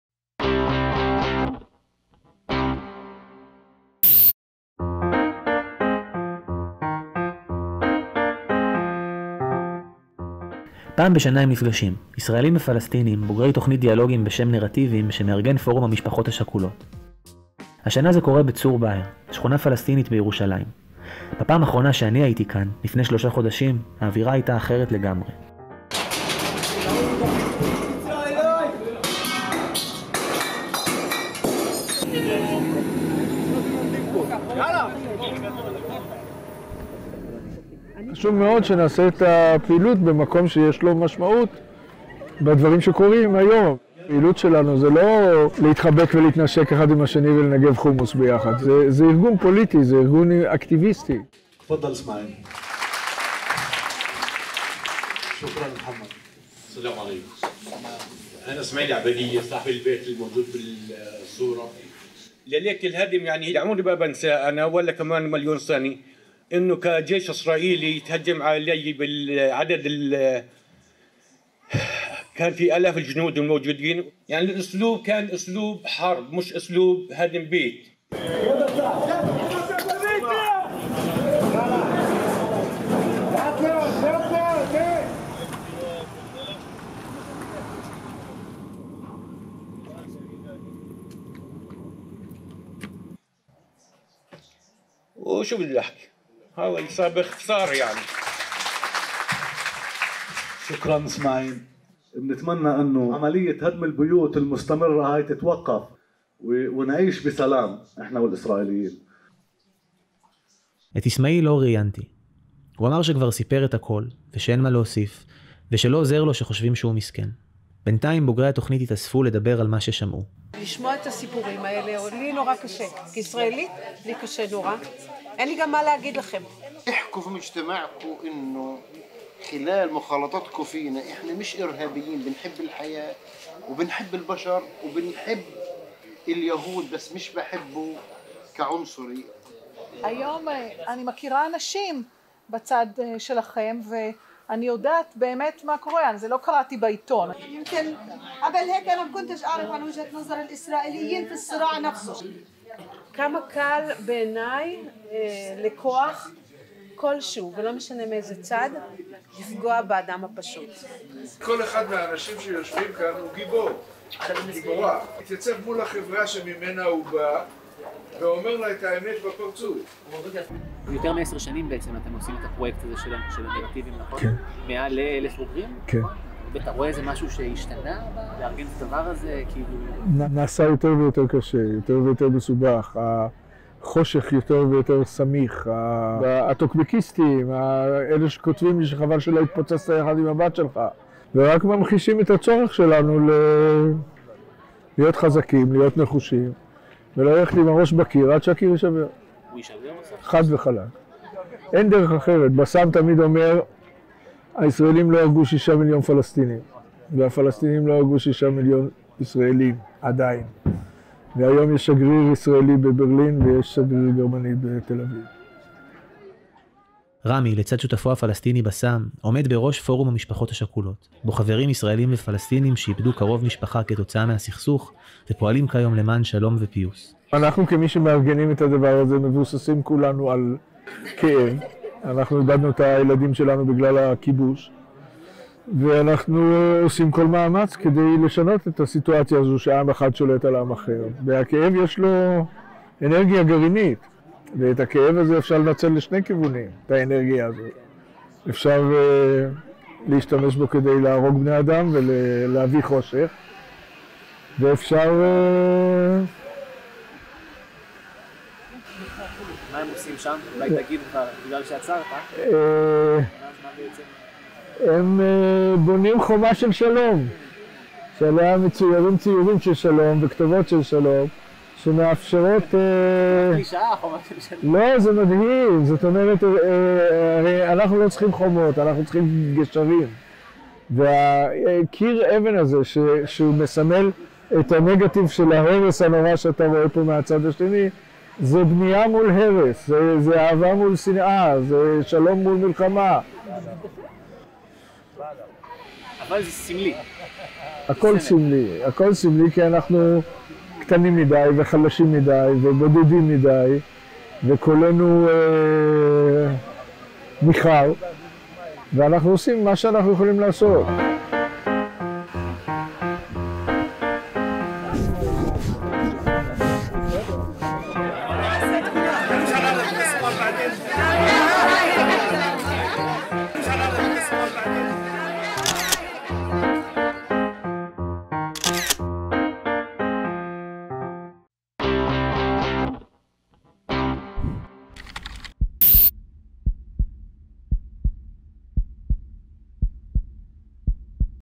בתגובה להריסת בתים: בונים חומה של שלום
מול הריסות הבתים בשכונת צור באהר במזרח ירושלים נבנתה חומה של שלום. הפעילות נערכה על ידי בוגרי תכנית הנרטיבים של פורום המשפחות השכולות הישראלי-פלסטיני בסולידריות עם תושבי השכונה.

אתם עמותה\ ארגון חברתי? צריכים לתעד כנס או לצלם סרטון תדמית לטובת המטרות שלכם?